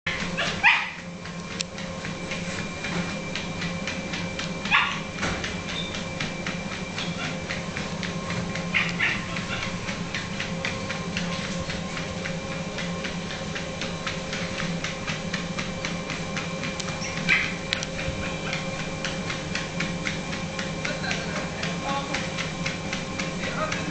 Soya tränar
Soya springer på löpbandet, andra gången! Duktiga Soya-Poya!